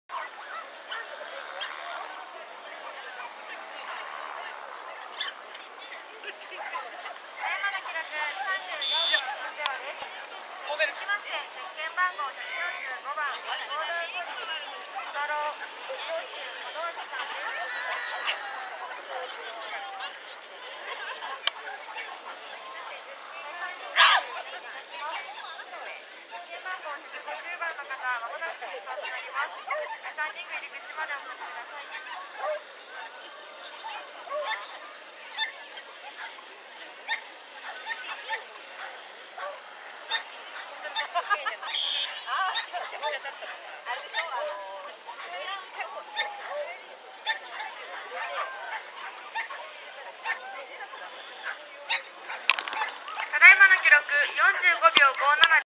2021/10/30 JKC JP3 AMI
ST連合会東日本アジリティー競技会@富士川緑地公園
スモールクラス
拒絶1(ハンドラーコース間違い)